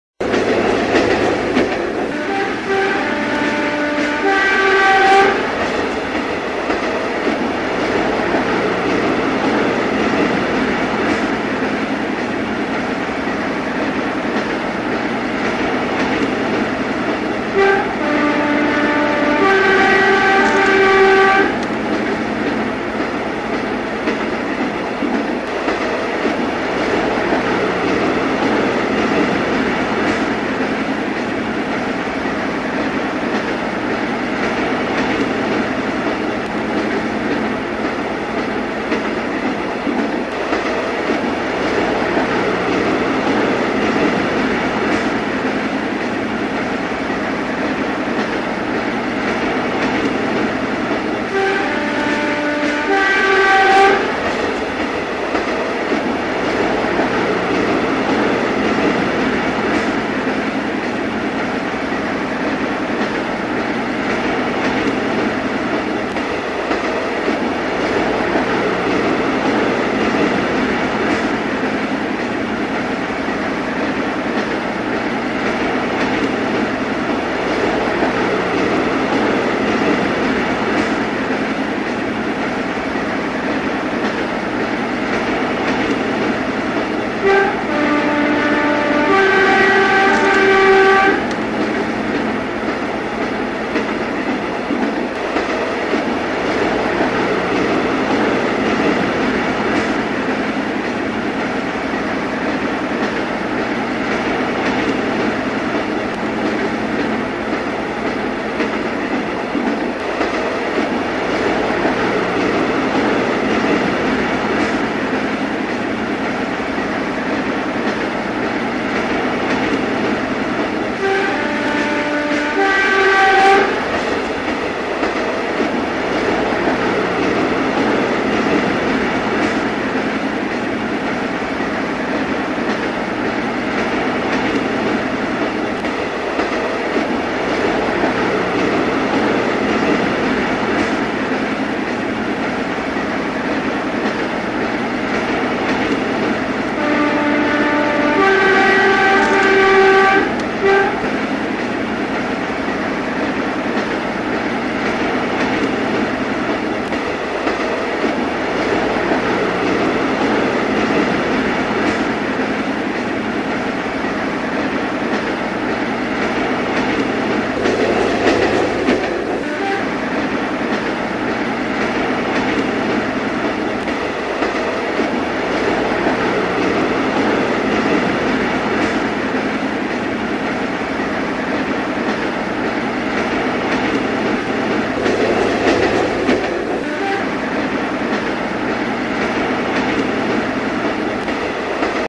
Espace de la Locomotive à Vapeur à OBERMODERN (67)
Cet espace a vu le jour grâce à un passionné, Monsieur FRAULI, citoyen d'Obermodern qui a réalisé ce travail fantastique et ce musée a vu le jour avec l'aide d'une vingtaine de bénévoles.